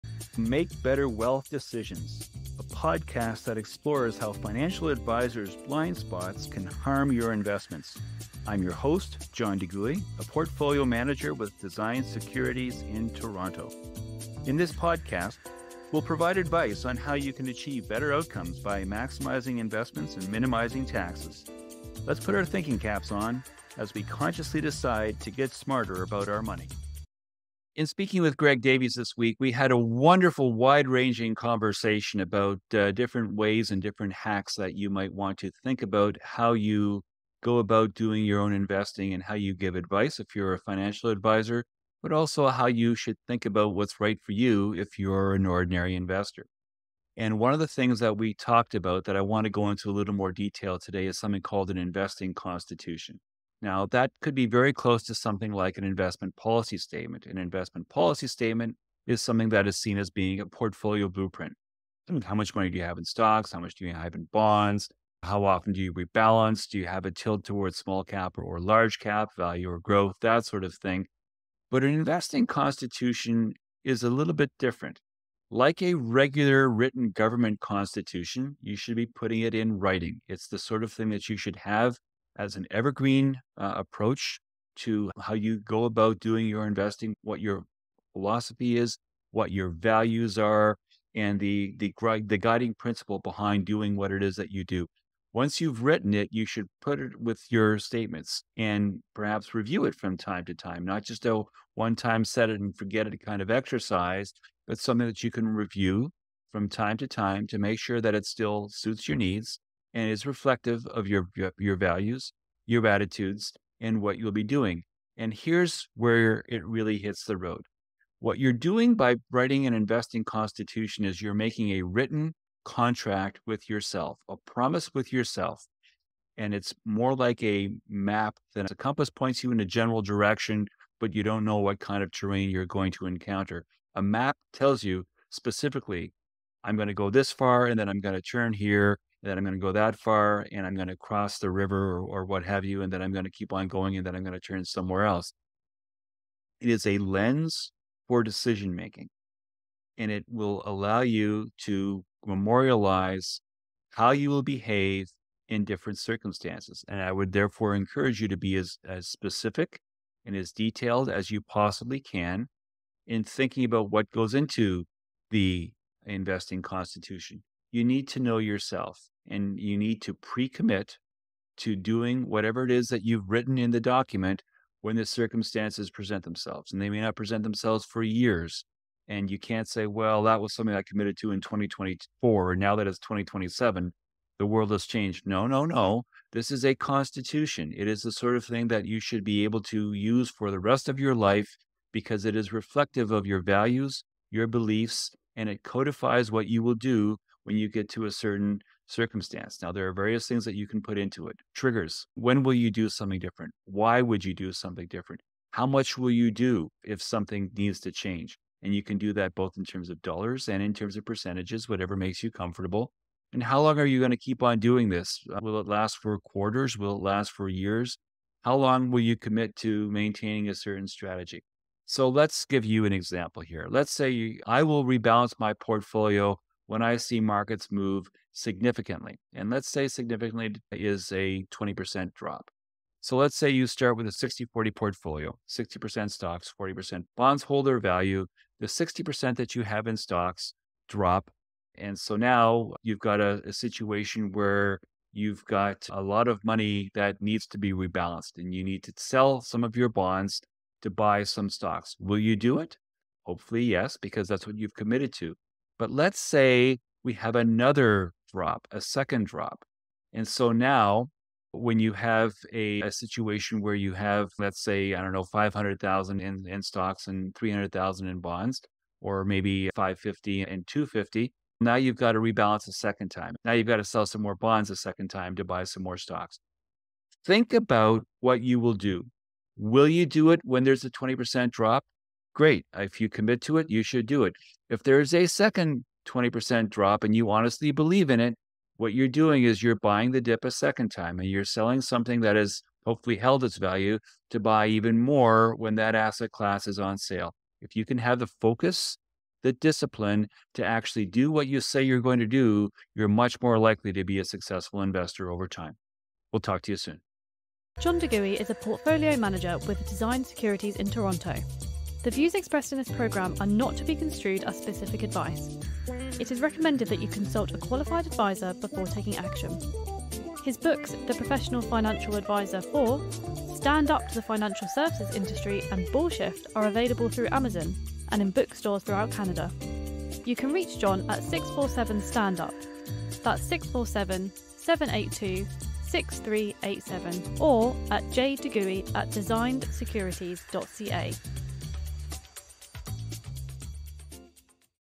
The Power of Financial Commitment Contracts
Have you ever wondered how a simple document could drastically improve your investment decisions and keep your emotions in check during market turbulence?

In today’s fast-paced financial world, it’s easy to get caught up in the chaos and make hasty investment decisions that may not align with your long-term goals.

This episode is tailored for those who want to bring order and clarity to their investment strategy, ensuring they stay true to their values and objectives regardless of market fluctuations.

By tuning in, you’ll discover the concept of an "investing constitution"—a personalized financial guide that helps you:

1) Clearly define and memorialize your investment philosophy and principles.
2) Establish triggers and rules for rebalancing your portfolio, so you’re never caught off guard.
3) Maintain discipline in your investment decisions, ensuring consistent success over the long term.

Don’t miss this opportunity to learn how to build your own investing constitution—press play now and take control of your financial future!